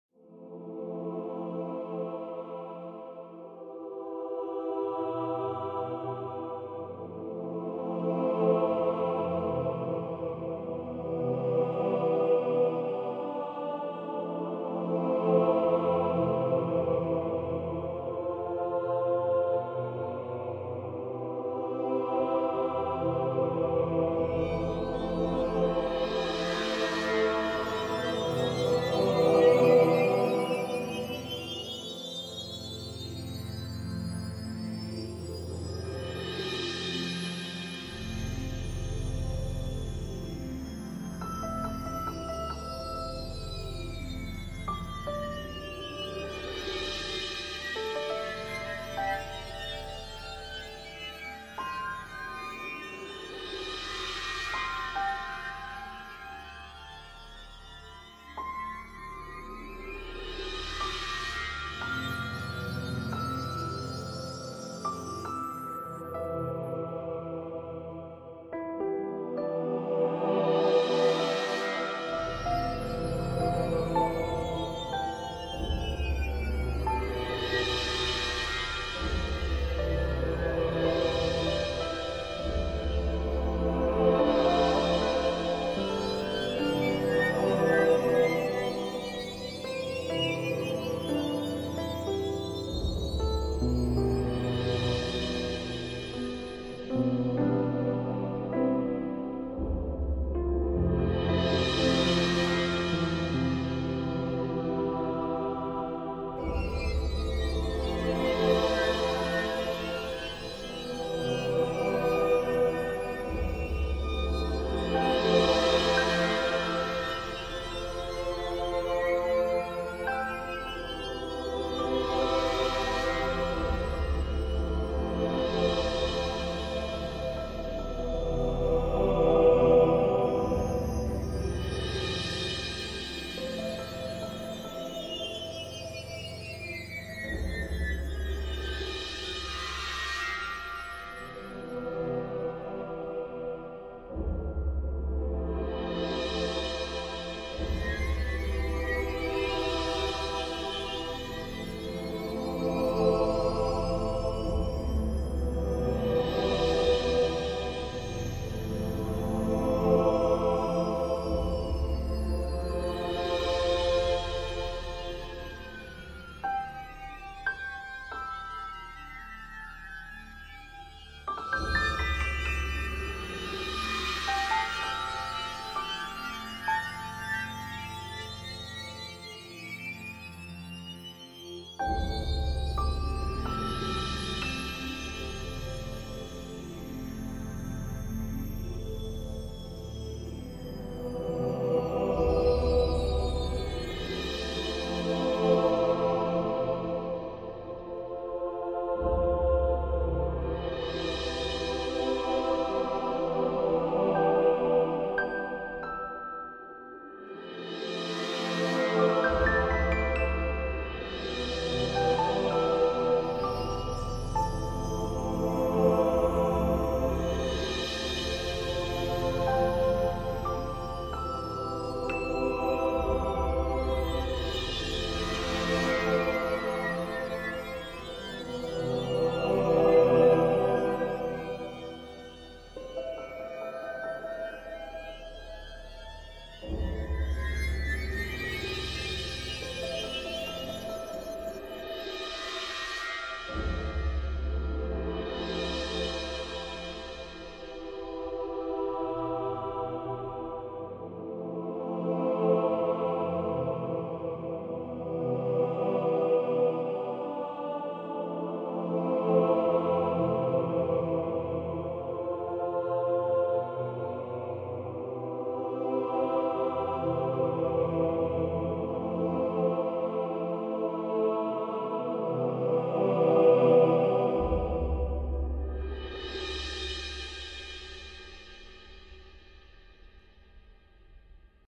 Music for relaxation, sleeping, meditation.
Music made by me. Ambient dreams.
No Copyrights and free to use If you make a link to my Channel.